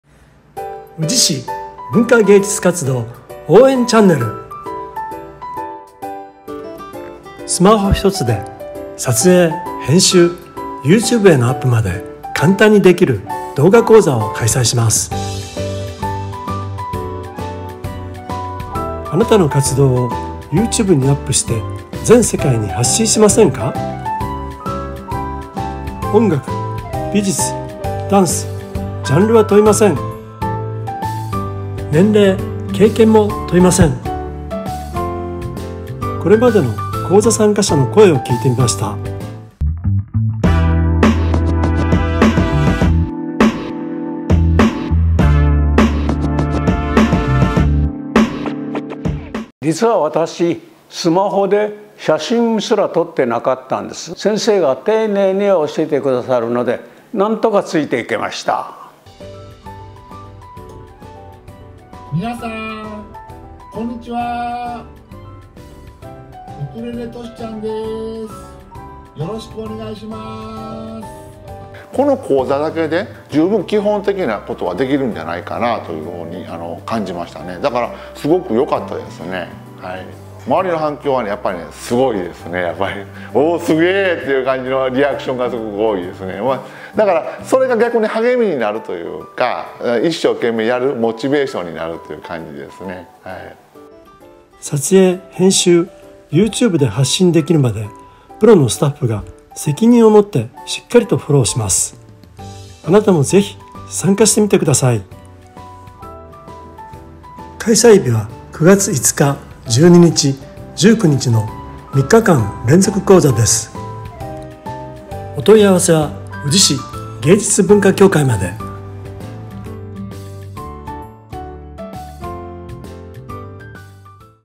令和６年度動画撮影・編集講座 参加者募集！
今年度も大人気のスマホ動画講座を開催します。９月５日（木）１２日（木）１９日（木）の３日間連続講座。スマホ一つで撮影・編集・YouTubeアップまで簡単に出来てしまいます。場所は宇治市生涯学習センター。参加無料。定員は20名。